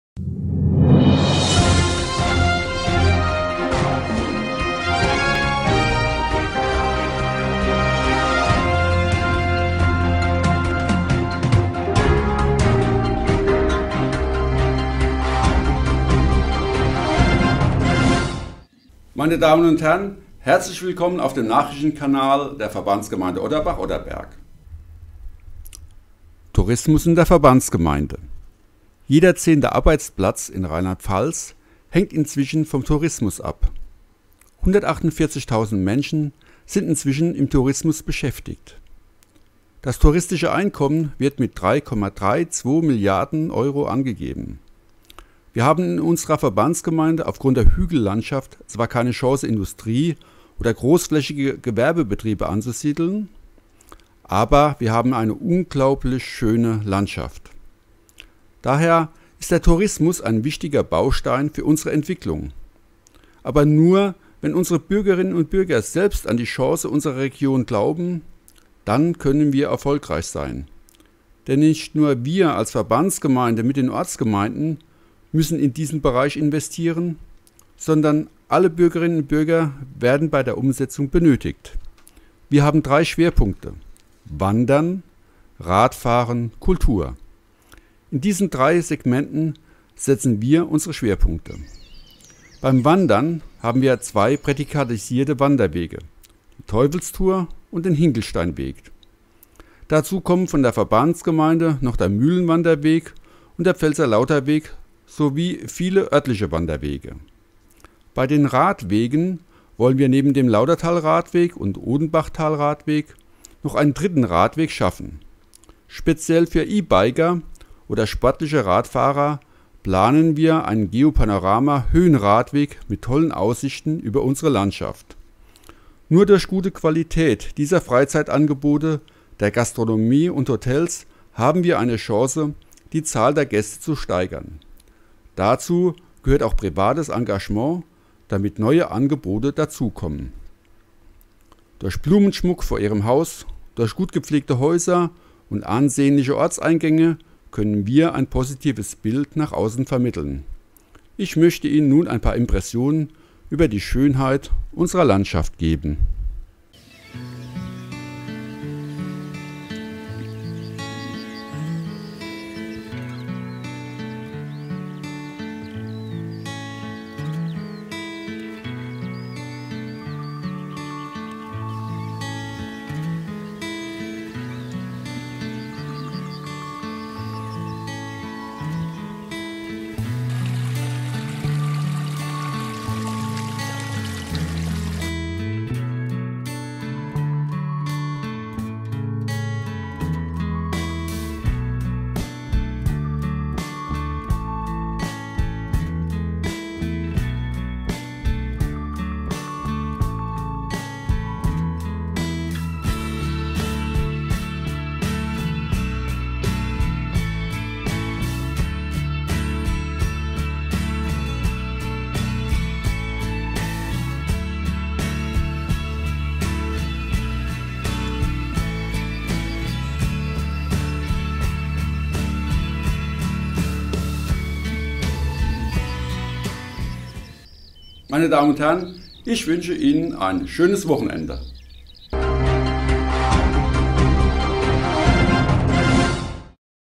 VG Otterbach-Otterberg Nachrichten KW23
Die Nachrichten der Verbandsgemeinde Otterbach-Otterberg haben für die KW 23 folgenden Themen:
- Tourismus in der Verbandsgemeinde